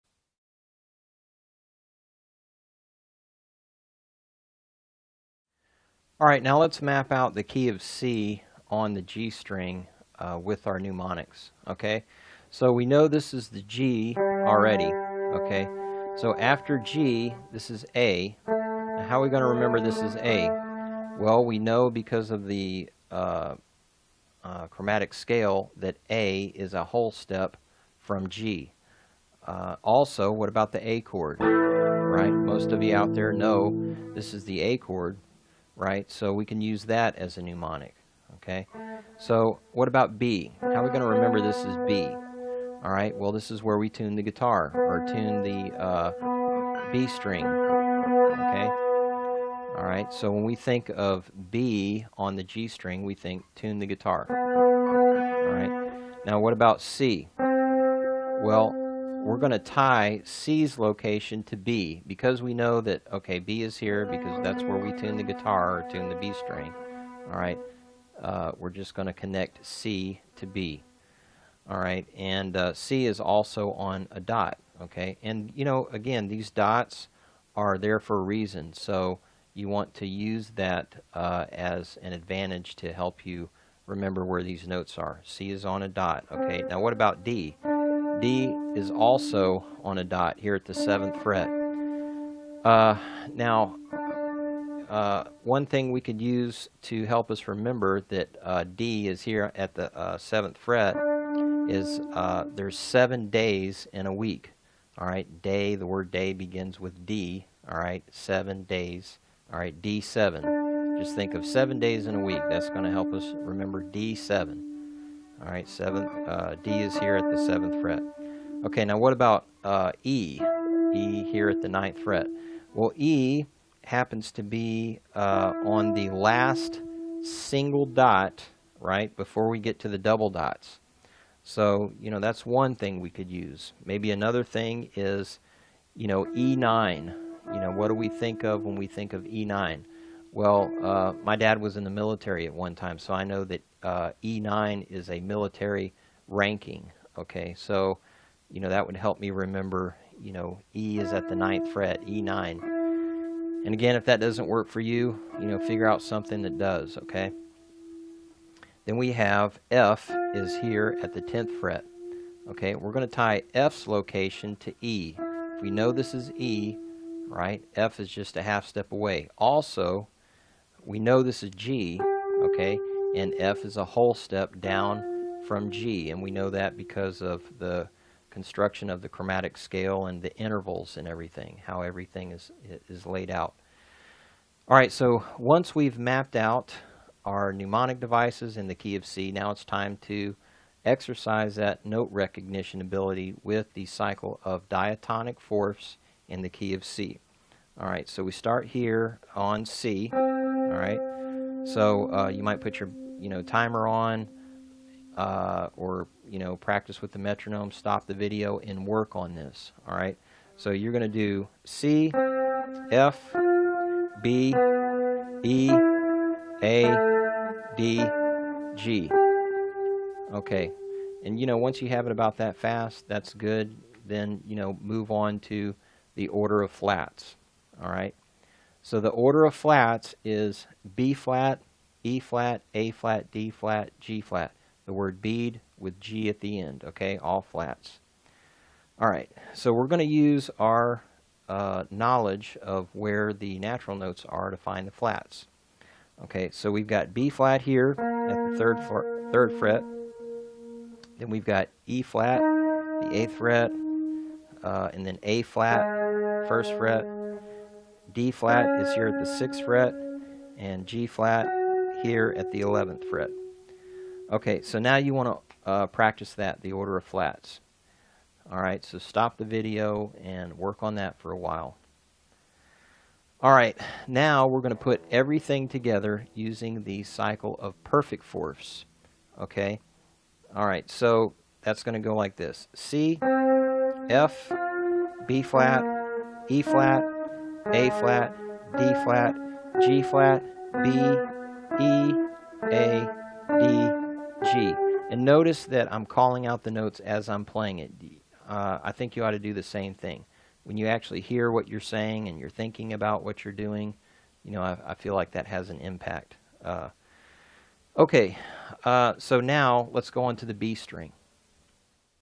Guitar Fretboard Positioning System | Chapter 7
In this video we will be mapping out the G string.  By following the method shown, with practice, you should be able to know where all the notes are without even looking at the guitar.